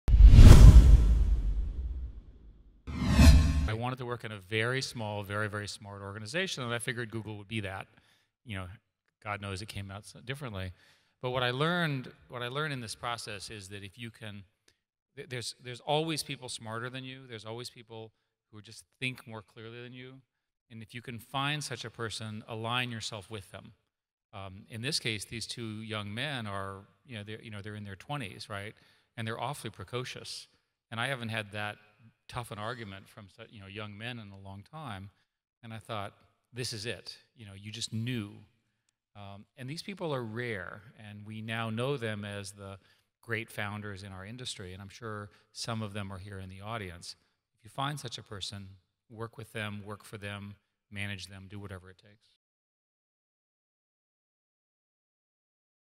ERIC SCHMIDT | WHY JOIN GOOGLE | STARTUP GRIND EUROPE
Why Eric Schmidt decided to join Google.